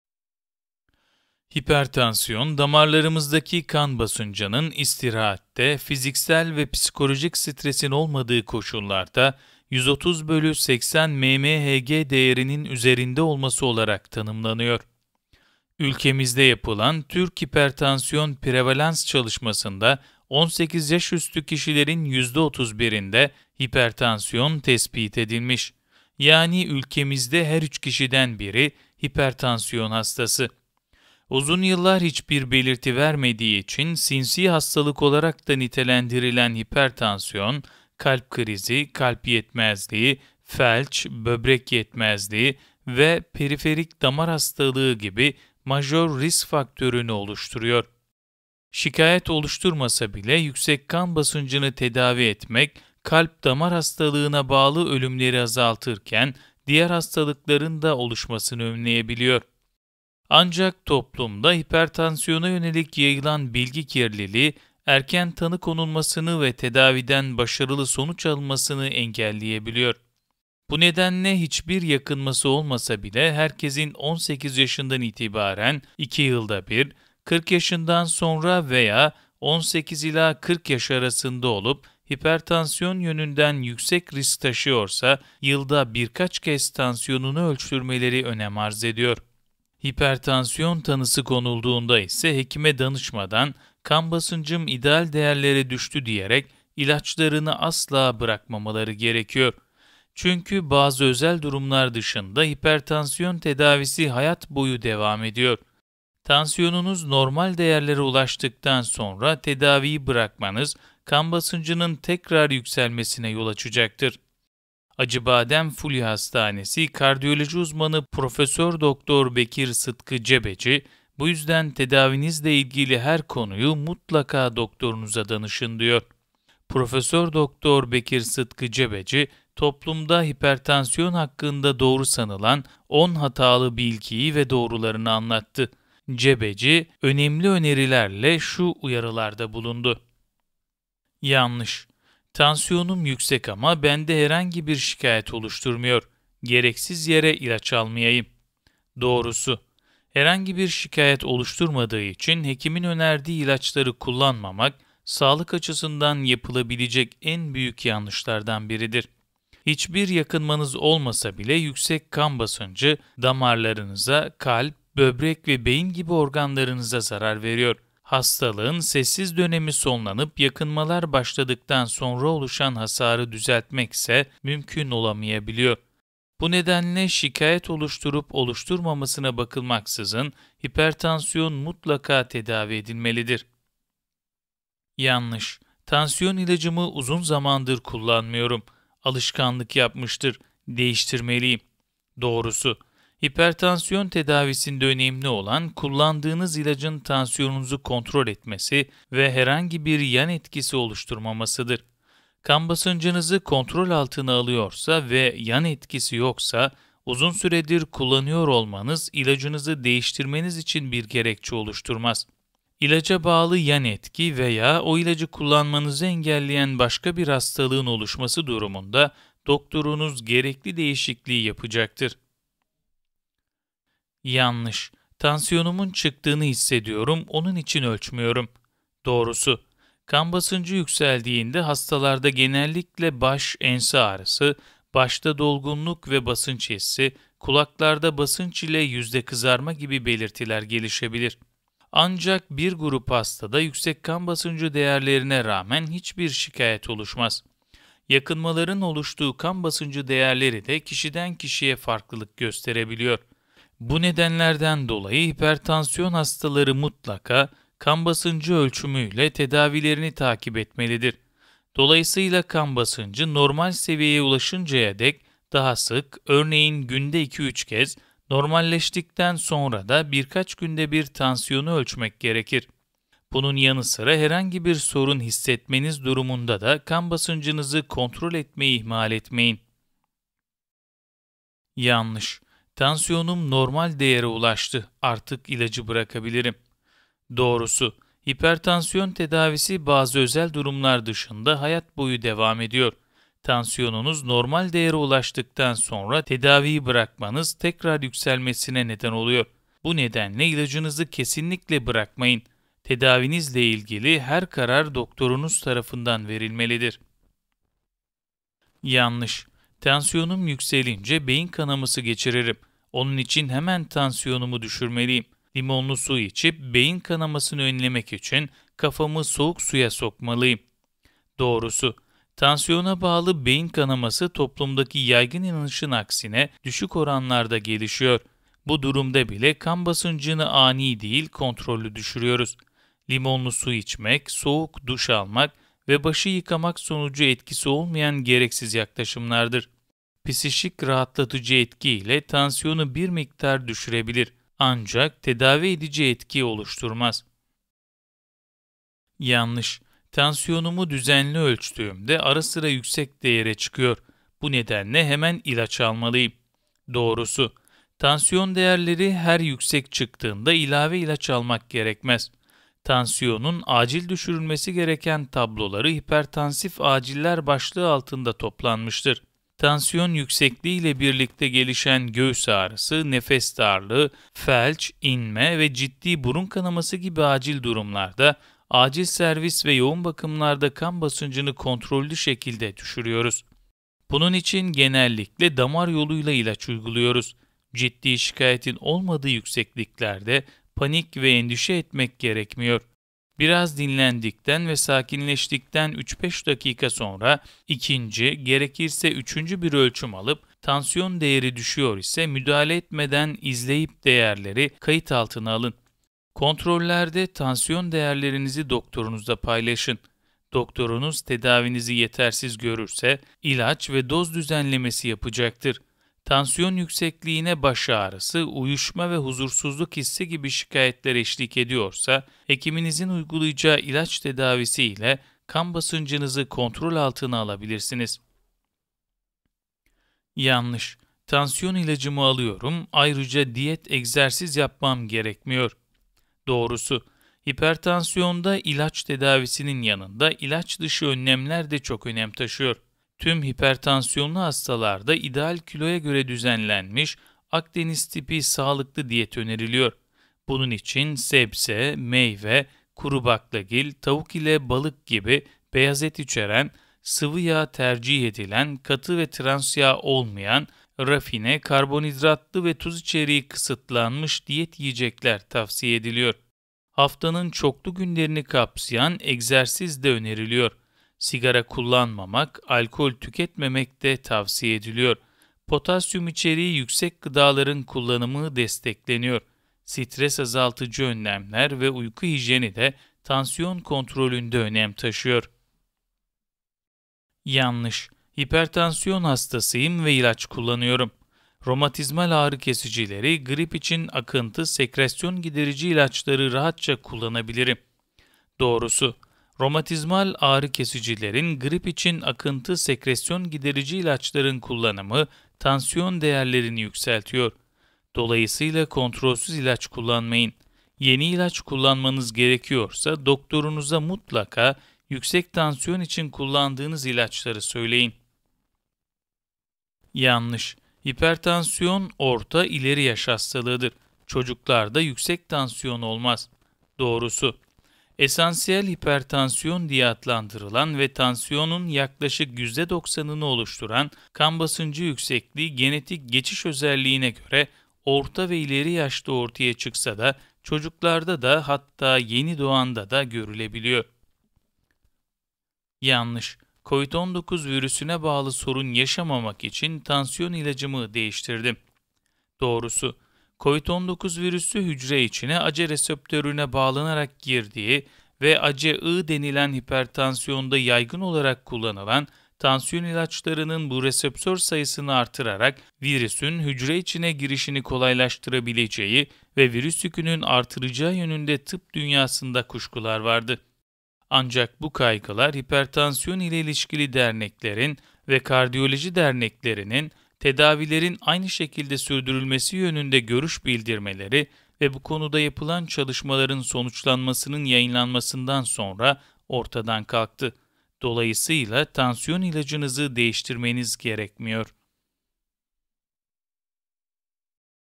Yüksek Tansiyon Hakkında Doğru Sanılan 10 Yanlış! Hipertansiyon nedir? Hipertansiyon belirtileri
Hipertansiyon, damarlarımızdaki kan basıncının istirahatte, fiziksel ve psikolojik stresin olmadığı koşullarda 130/80 mmHg değerinin üzerinde olması olarak tanımlanıyor. Ülkemizde yapılan Türk Hipertansiyon Prevalans çalışmasında; 18 yaş üstü kişilerin yüzde 31’inde ‘hipertansiyon’ tespit edilmiş. Yani, ülkemizde her 3 kişiden biri, hipertansiyon hastası! Kardiyoloji Uzmanı Prof. Dr. Bekir Sıtkı Cebeci toplumda hipertansiyon hakkında doğru sanılan 10 hatalı bilgiyi ve doğrularını anlattı; önemli öneriler ile uyarılarda bulundu.